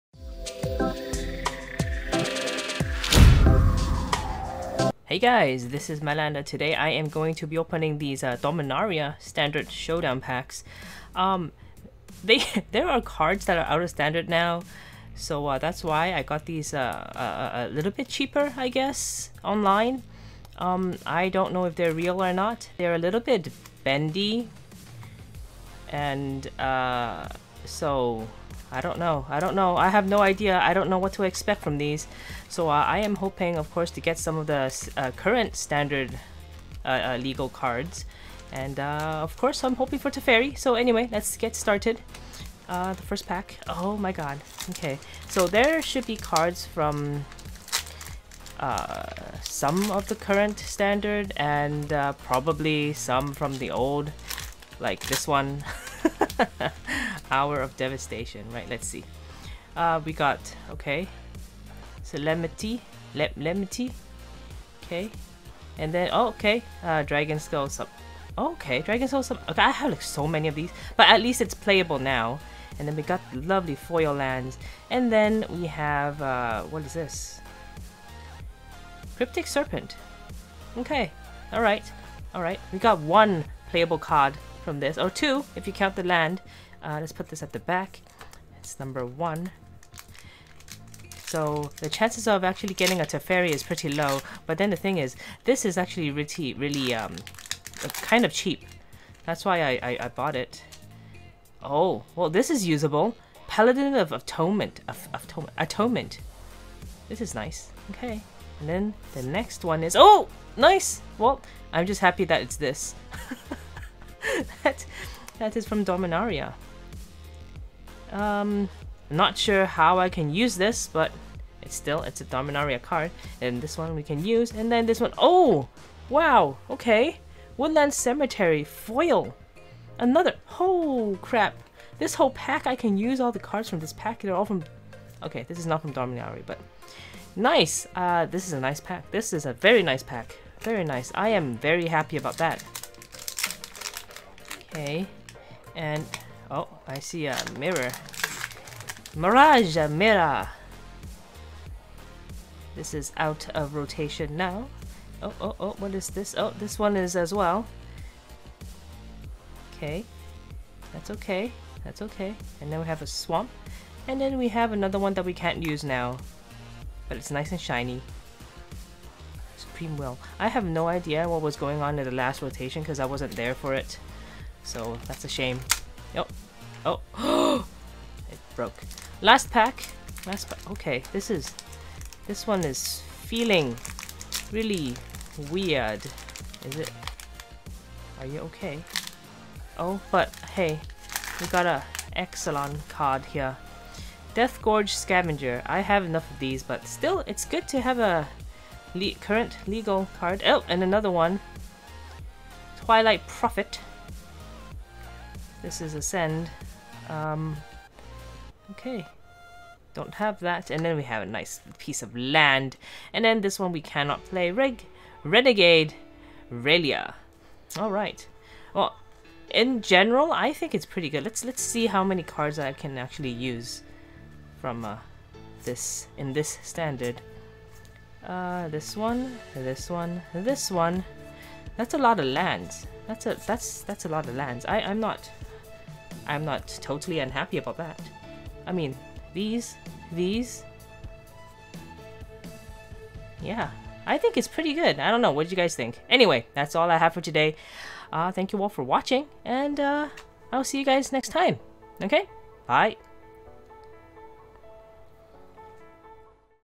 Myl Opens DOMINARIA STANDARD SHOWDOWN PACKS X4
Got these Dominaria Standard Showdown packs for relatively cheap since most of the cards are out of rotation. Hoping to get some standard legal cards in these! Cross your fingers!